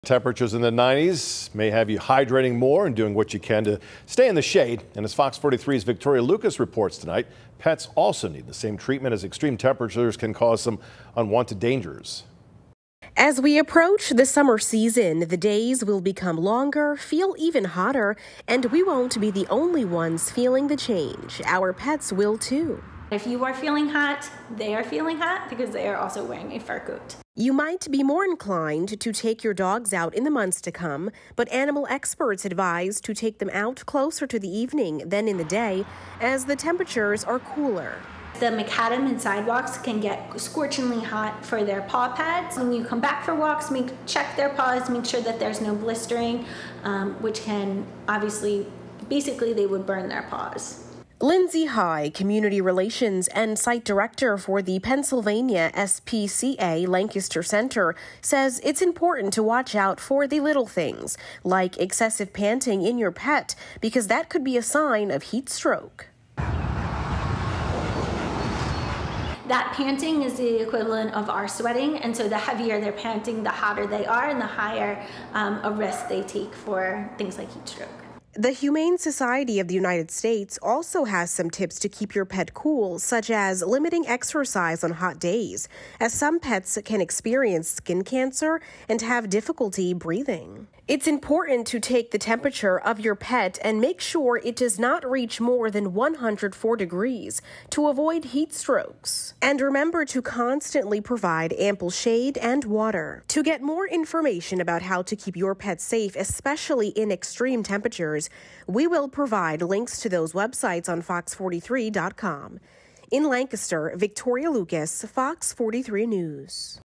Safety tips for pets during extreme heat
As temperatures begin to rise, the need for more water and shade will too.

However, people won't be the only ones yearning for this- animals will as well.

"It's really important to keep in mind that if you are feeling hot, they are feeling hot because they are also wearing a fur coat," said Lindsay High, community relations and site director for the Pennsylvania SPCA Lancaster Center